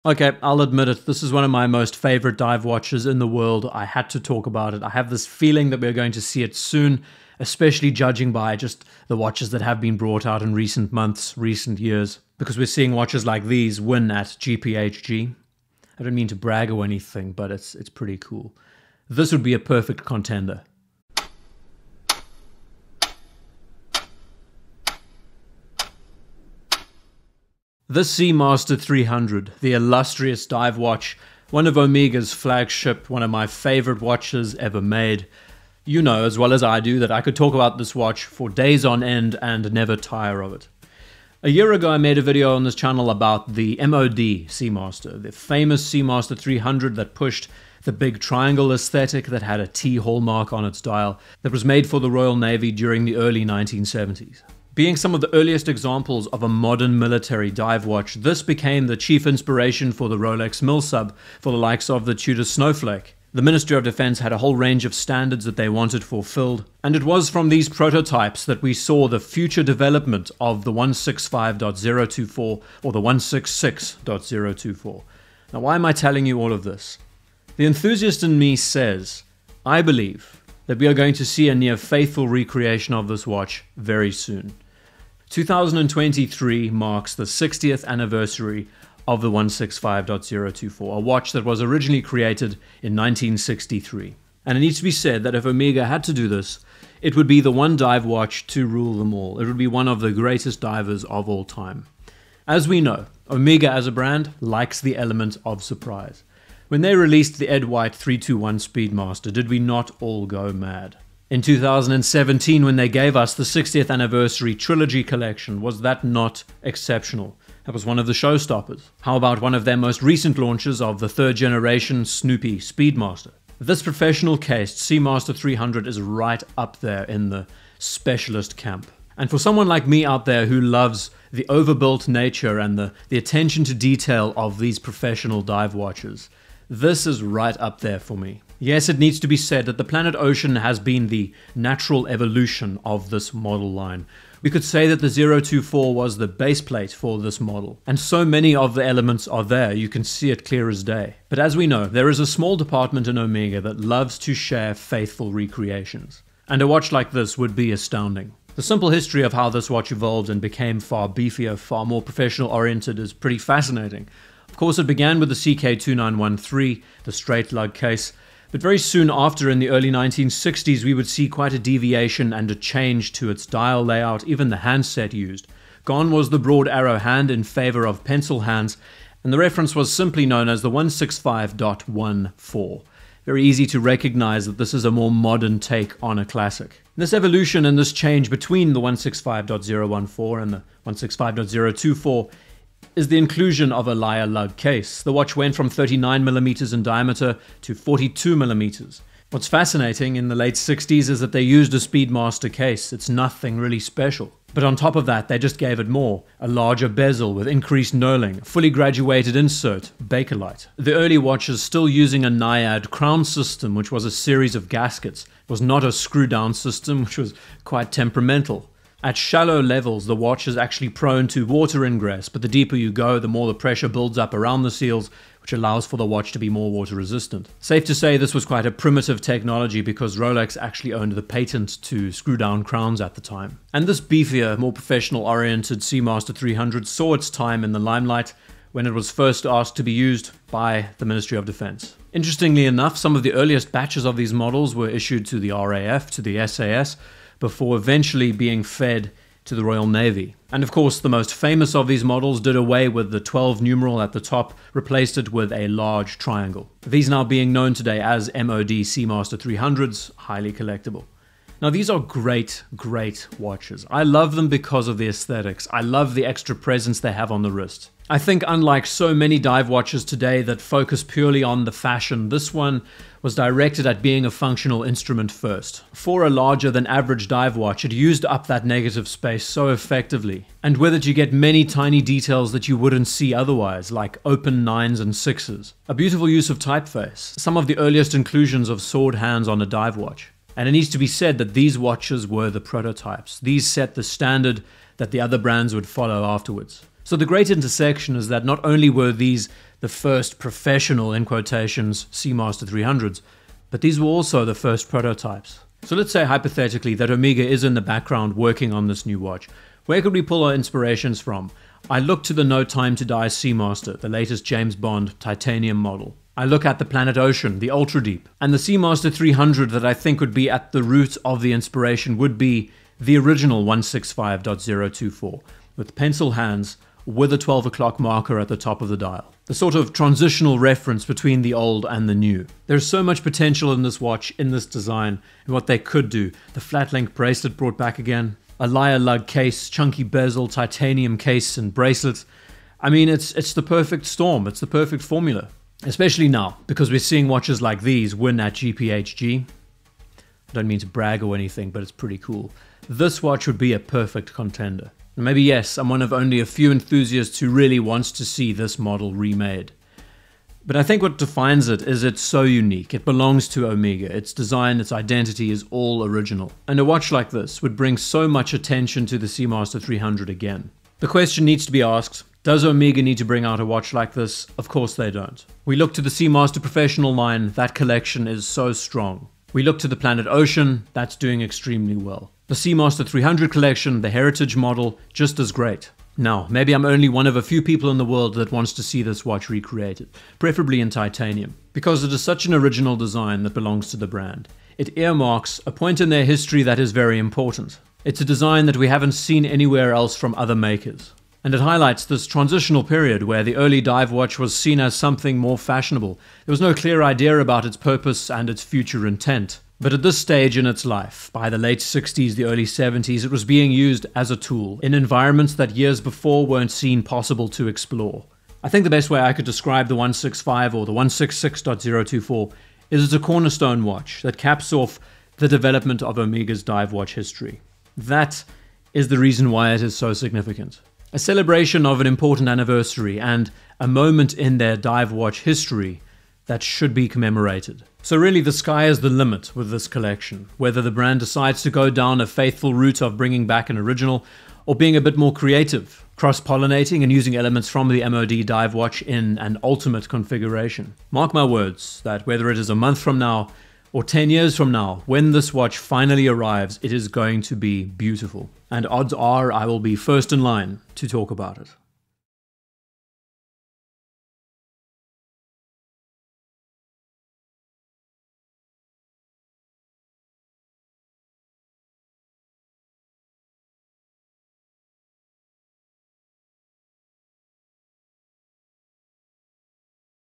Omega's Expected Release of the Seamaster 300 Anniversary (165.024)
A Discussion around the expected Release and Return of the Omega Seamaster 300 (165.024) with an upcoming 60th Anniversary and a Heritage department that enjoys sharing faithful recreations, I strongly believe that we will be seeing this watch return soon as a Special Edition. Like the Ed White Speedmaster and the '57 Trilogy, the revival of the Original Professional Omega Seamaster would be a game-changer.

Intro 0:00
Omega Release Surprises 1:42
Seamaster 300 Evolution 2:39
Seamaster 300 Specs 3:19
Seamaster Design Ideas 4:50
Seamaster Renders 5:32
Conclusion 6:11

All Images are Sourced from either Press Release Photos or Public Listings.
Below are a list of the common sources that are used in no particular order: